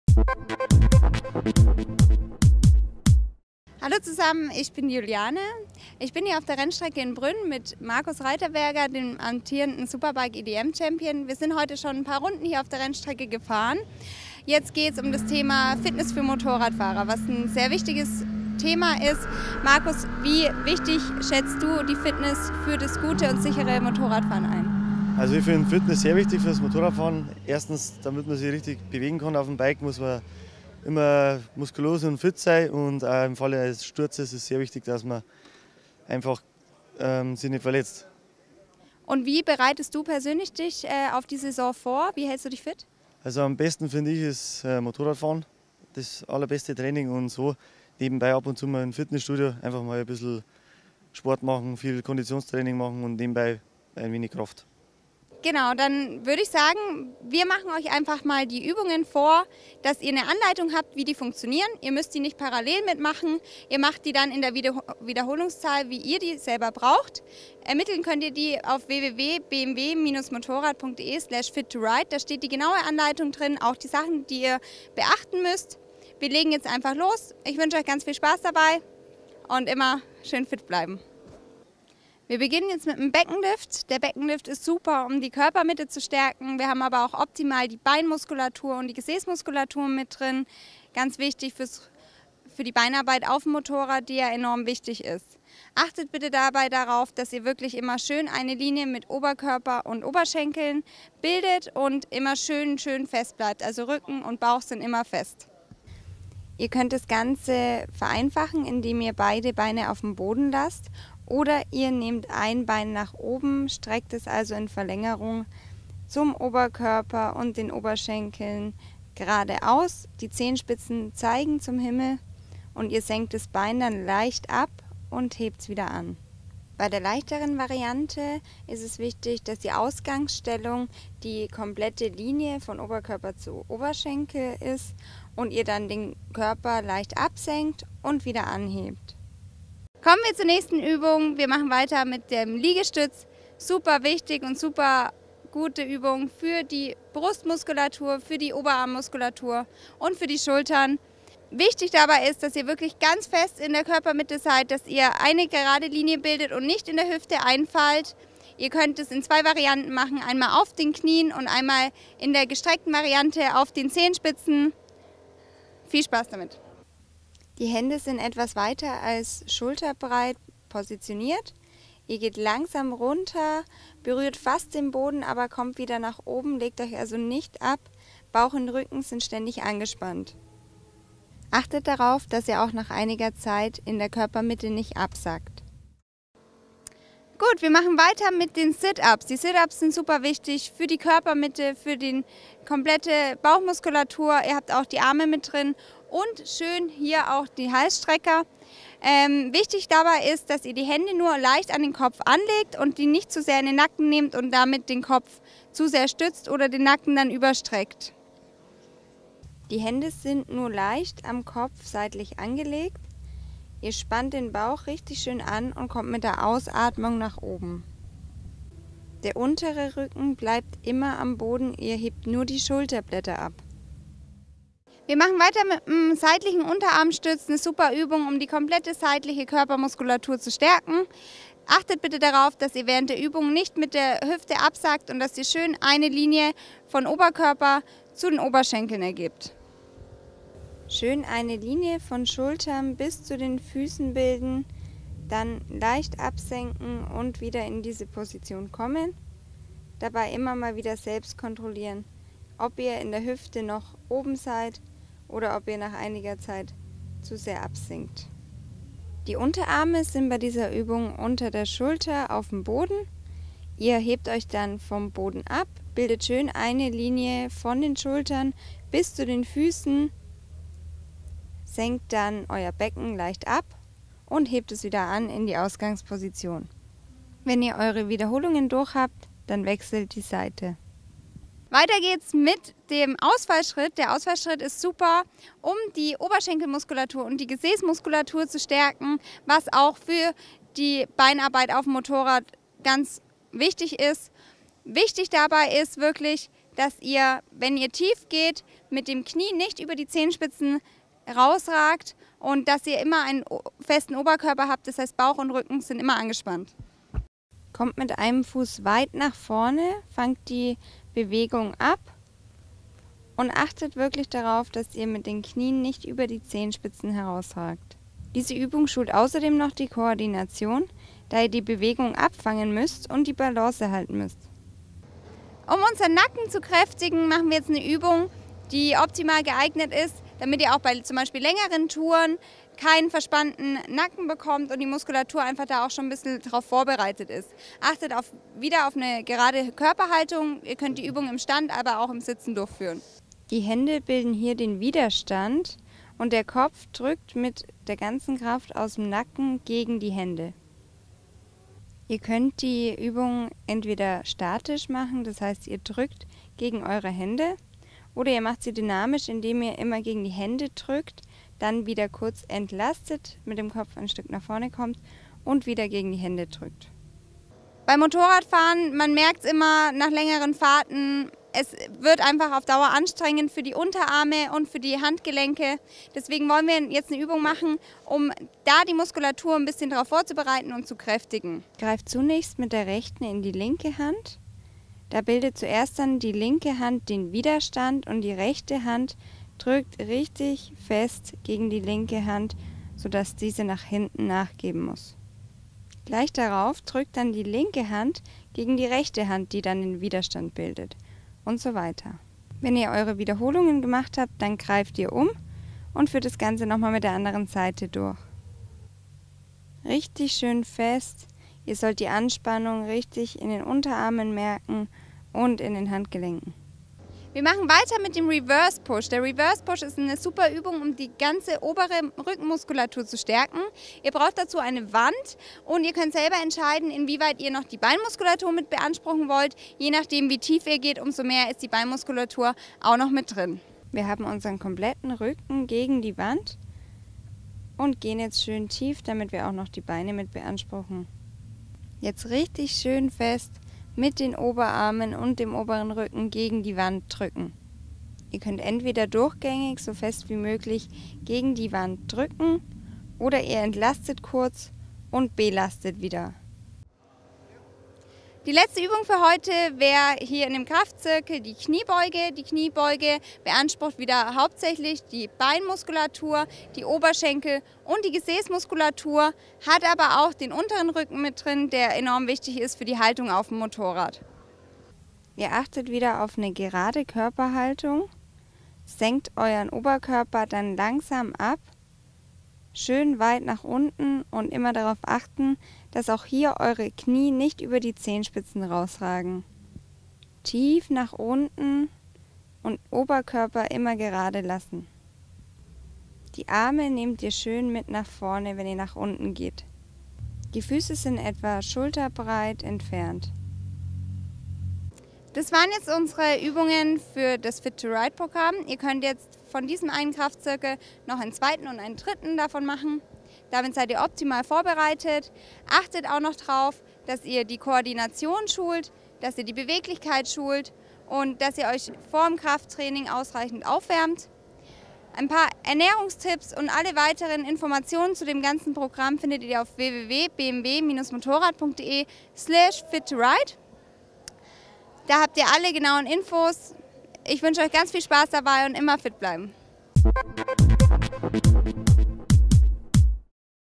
BMW Motorrad Fit2Ride Trainingsprogramm - Fitnesstraining
Motorradfahren ist Sport. Um gut und sicher unterwegs zu sein, bedarf es einer ausgebildeten körperlichen sowie geistigen Fitness, denn eine frühzeitige Ermüdung kann den Reaktionsprozess verzögern und die Konzentration stark einschränken.
Wer seine Fähigkeiten beim Motorradfahren verbessern will, sollte mit dem Training nicht erst auf der Maschine beginnen, sondern schon im Vorfeld an seiner Fitness arbeiten.
Mit dem Fit2Ride Trainingsprogramm gibt BMW Motorrad jetzt erstmals Fitnessempfehlungen zur Verbesserung von Kraft, Koordination, Ausdauer und Beweglichkeit speziell für Motorradfahrer. Das Fit2Ride Trainingsprogramm wurde speziell auf die Belastungen beim Motorradfahren abgestimmt. Sowohl Anfänger als auch fortgeschrittene Sportler können von dieser Trainingsempfehlung profitieren, da es individuell an jeden körperlichen Trainingszustand angepasst werden kann.